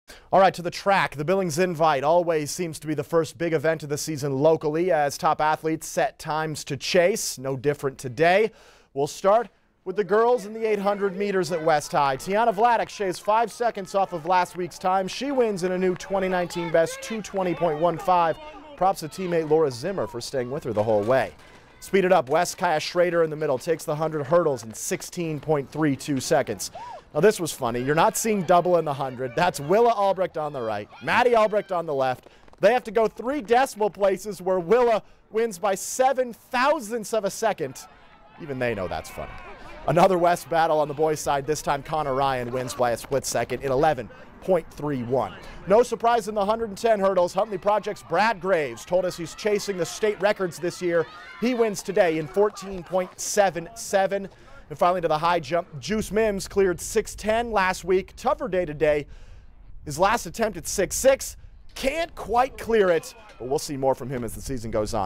New state-best times highlight Billings Invite meet
Several state-best marks were set Saturday at the Billings Invite track and field meet, including both girls' relay races plus an early top time from Billings Senior distance star Tiahna Vladic.